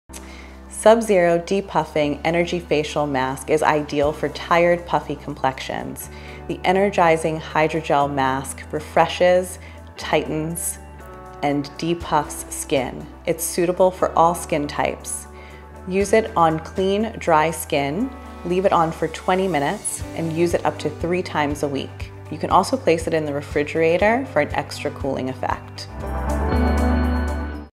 SUB-ZERO DE-PUFFING ENERGY FACIAL MASK
The Sub-Zero De-Puffing Energy Mask is perfect for tired, puffy and lacklustre complexions. It is an energising treatment mask designed to tighten, de-puff and refresh tired, puffy-looking skin.

The Sub-Zero De-Puffing Energy Facial Mask is a two-piece, full face hydrogel mask formulated to alleviate signs of puffiness and fatigue. Infused with peptides, enzymes and caffeine, the multi-functional formula includes a powerful tetrapeptide to help reduce puffiness, hydrate and tighten. The complexion is left looking refreshed and reinvigorated.